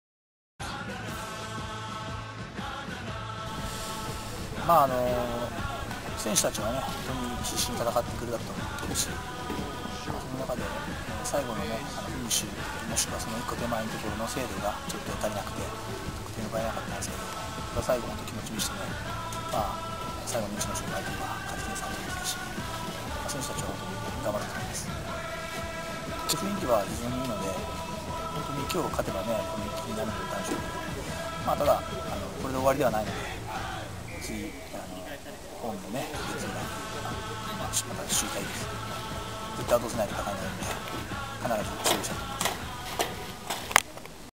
RENOFA.TV 中国サッカーリーグ第８節 試合後監督インタビュー
レノファ山口ー松江CITY．FC （１－１）月岡監督インタビュー